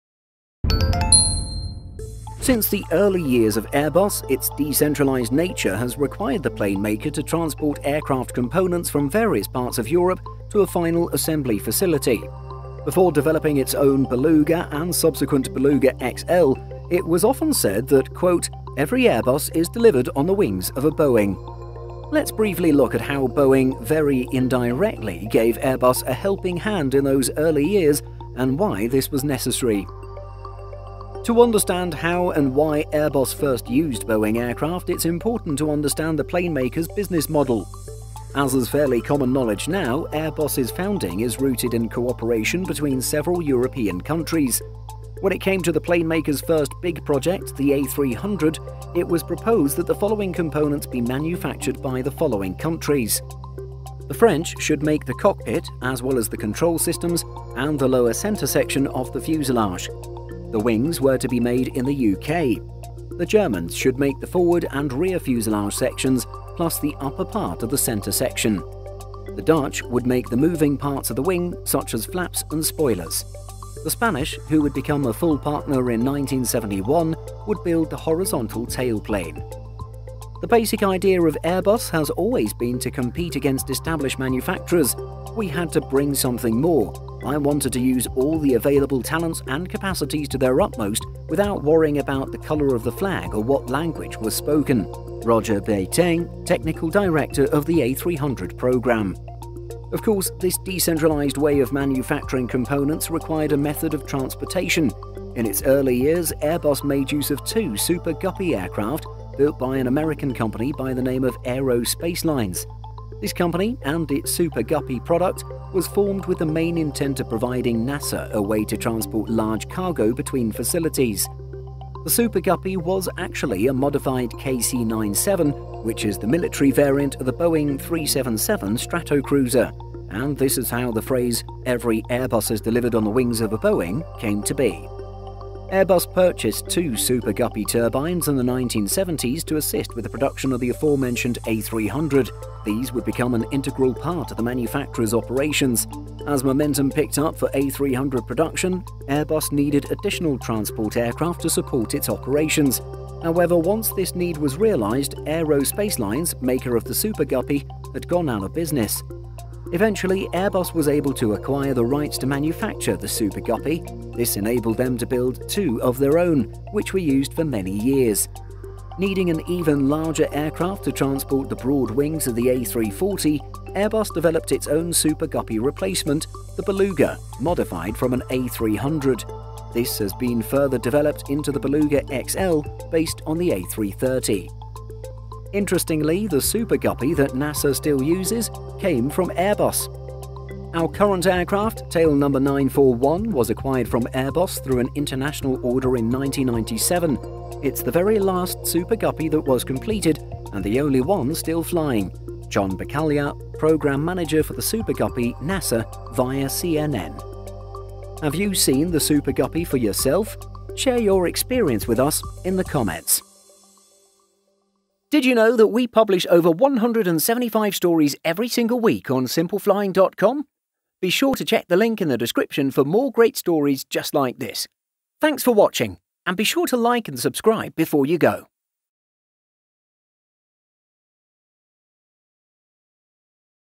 Why Airbus Used To Carry New Aircraft Parts In Boeing Aircraft
Since the early years of Airbus, its decentralized nature has required the planemaker to transport aircraft components from various parts of Europe to a final assembly facility. Before developing its own Beluga and subsequent Beluga XL, it was often said that quote “every Airbus is delivered on the wings of a Boeing.”

Let’s briefly look at how Boeing (very) indirectly gave Airbus a helping hand in those early years and why this was necessary...